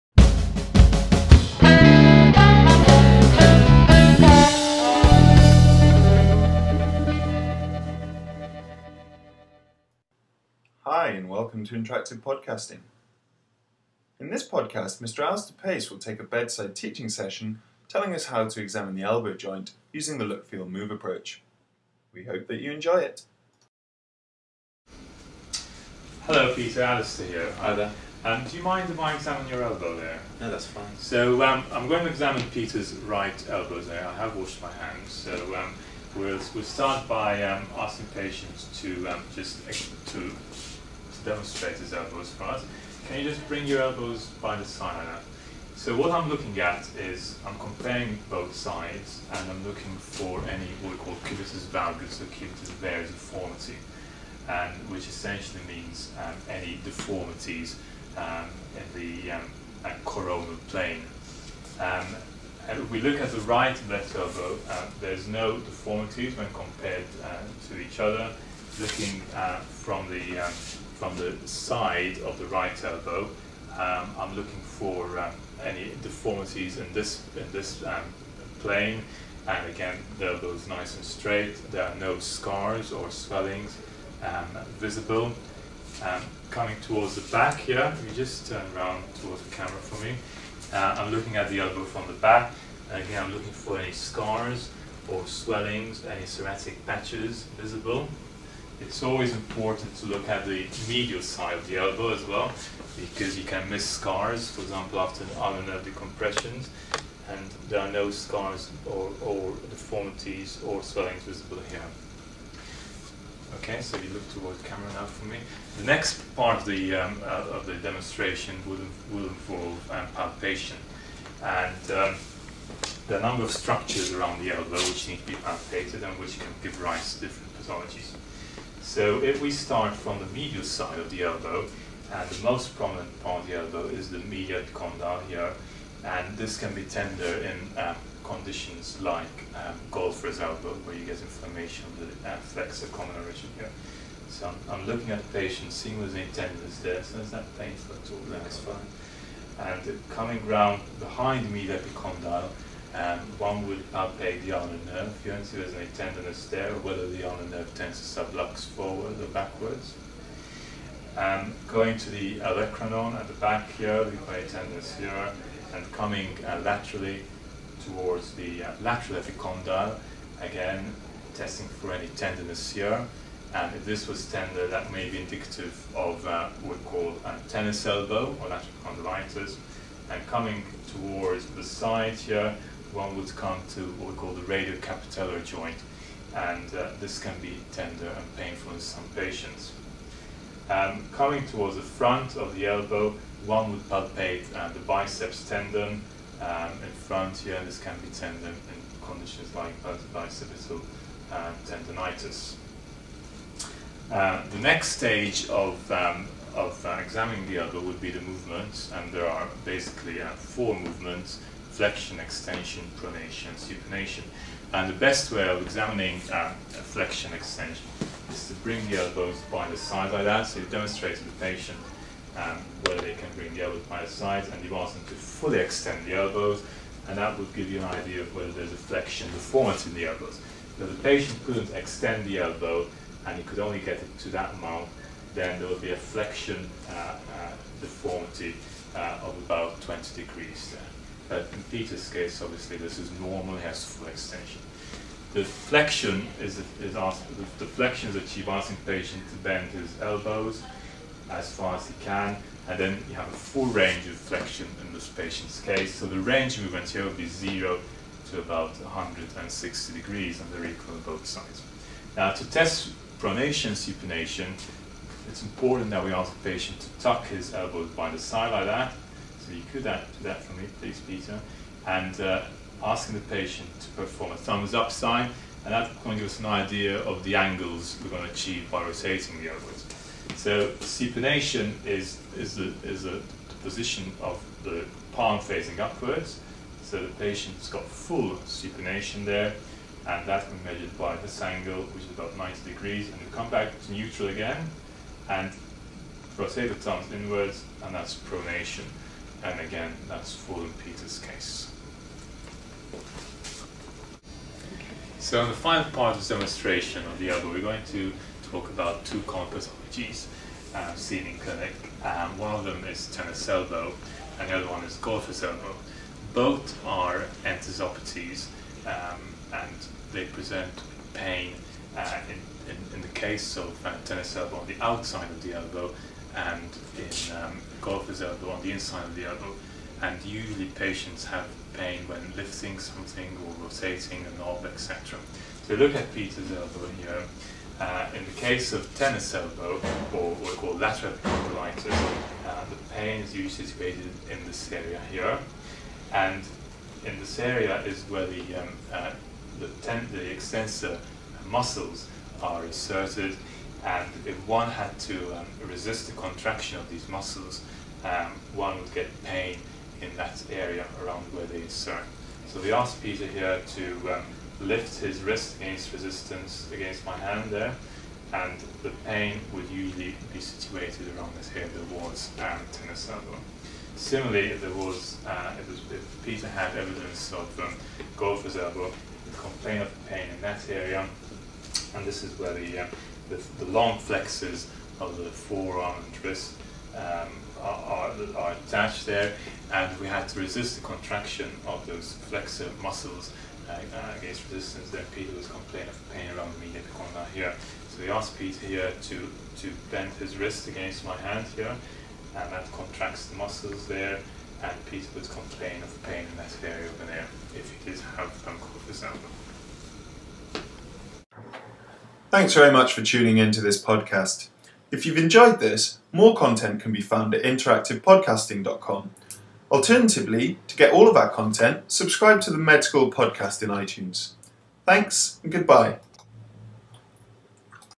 Elbow Examination
In this video Mr. Pace demonstrates how to examine the elbow at the bedside using the Apley system of examination ( Look, Feel, Move ). The video goes on to explain how to examine for the common conditions of Tennis and Golfers Elbow.